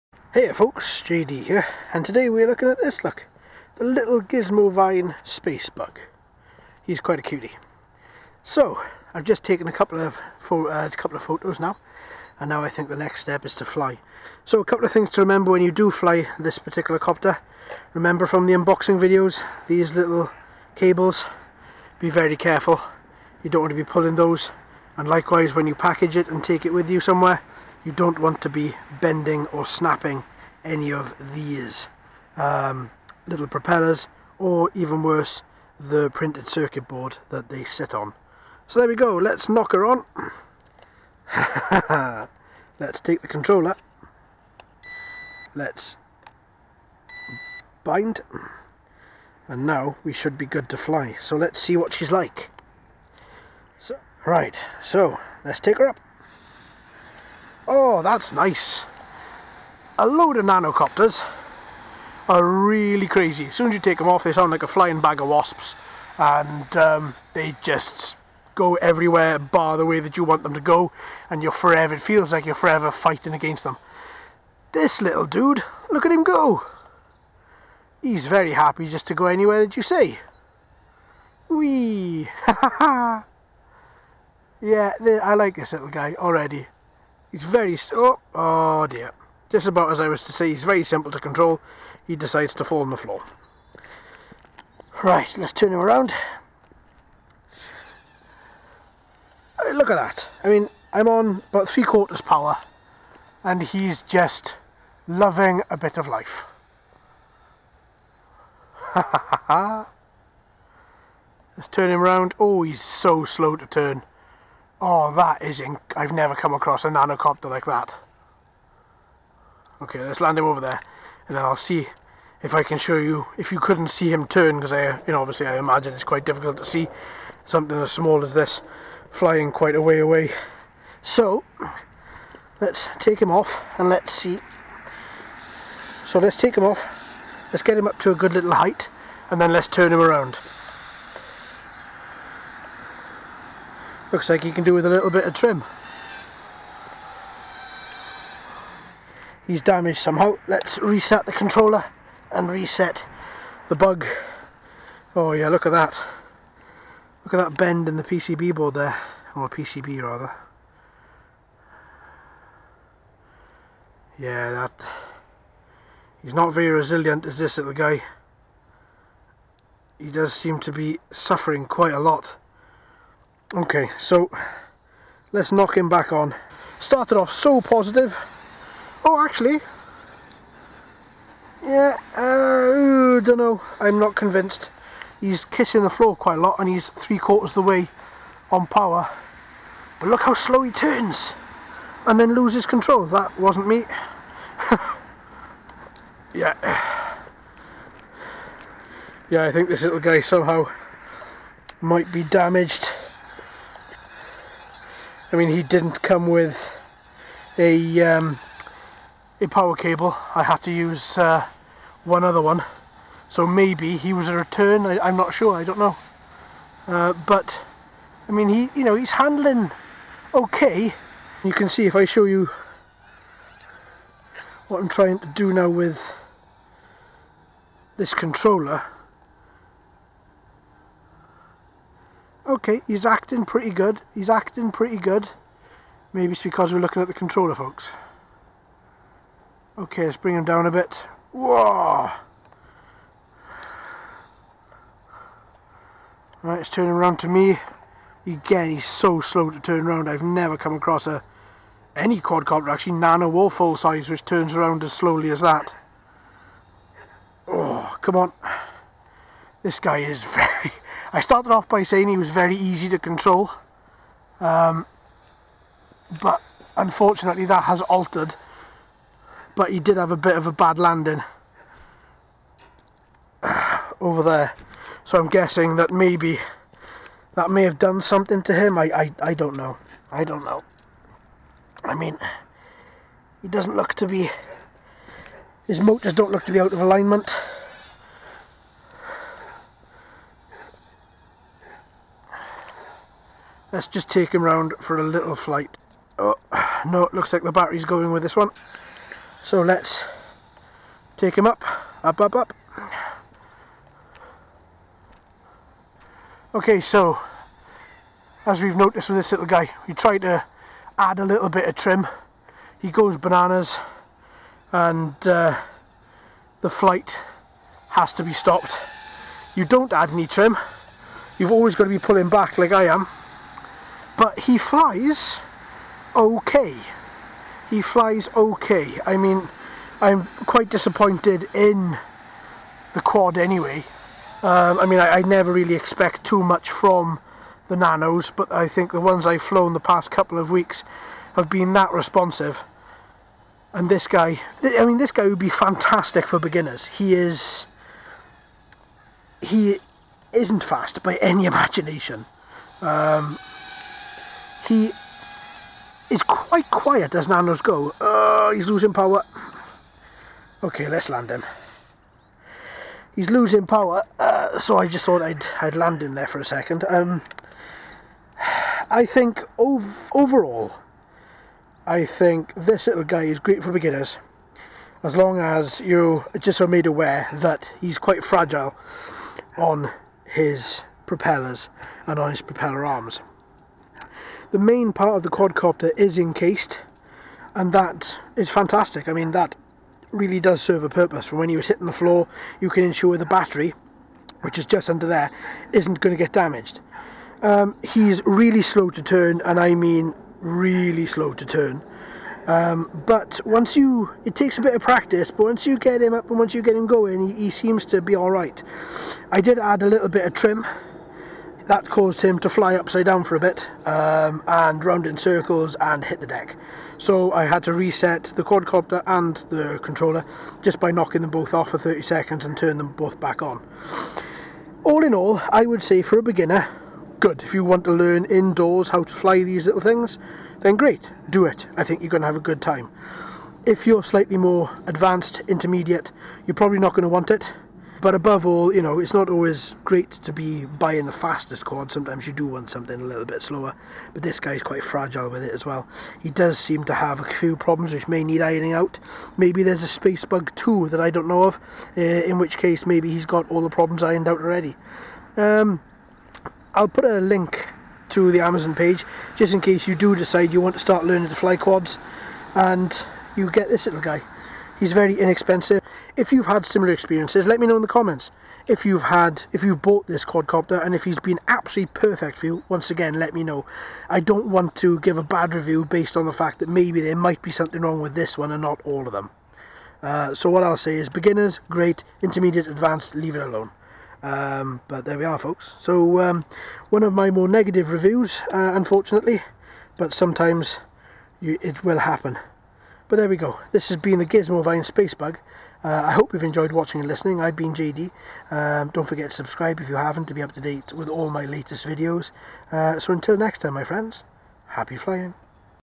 SPACEBUG GT-JZ003-U Quadcopter Drone Flight Test Review
SPACEBUG GT-JZ003-U is a nano quadcopter drone that styles itself on an insect or bug.  In this flight test review we look at its handling, flight and how this new style transmitter holds up.

Mobius Camera =
RunCam R/C Camera =
DBPower Camera =